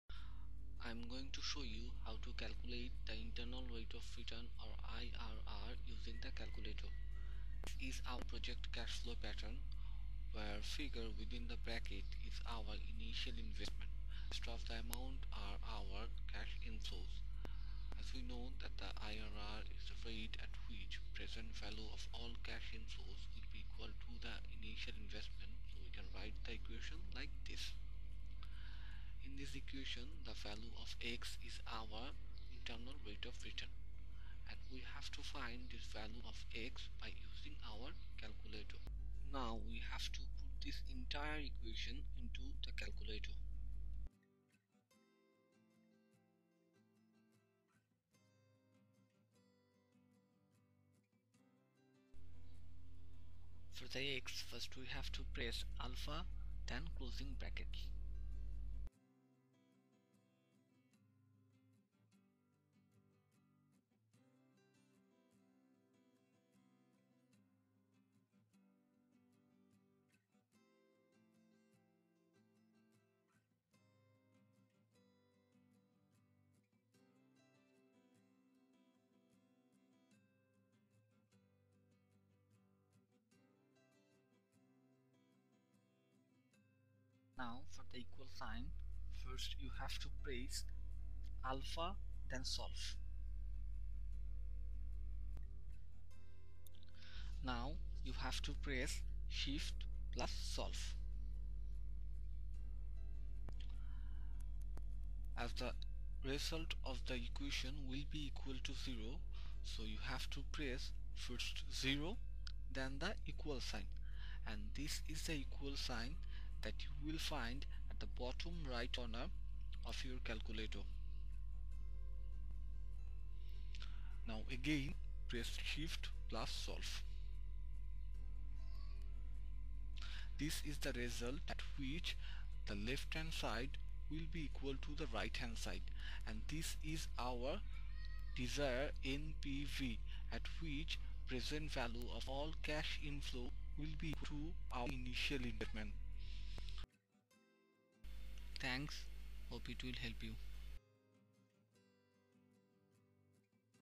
Internal rate of return or IRR using Casio fx claculator
This is the way to calculate the IRR using Scientific Calculator.
In the cash flow pattern we can see that 3500 is the initial investment at the beginning of year-1 (at year-0) and rest of the amounts are the cash inflows though out the project's lifetime. IRR is the rate at which present value of all cash inflows are equal to the Initial investment.

(2000/〖(1+X)〗^1) +(1500/〖(1+X)〗^2) +(1000/〖(1+X)〗^3) -3500=0

So we set the equation of present value of all cash inflows considering the unknown rate (IRR) represented by ‘X’. Then we subtract the initial investment from the sum of present value of all cash inflows and set the equation equal to Zero. The IRR is the value of ‘X’ in which equation value is ‘Zero’.

First we have to write the equation in the calculator. For equal sing (=) we have to press ALPHA+CALC.
After putting the whole equation we have to press SHIFT+CALC (solve).
As the other side would be equal to zero, we have to press ‘0’ (zero) then press ‘=’(which is in the bottom right corner of the calculator). Then again SHIFT+CALC (solve).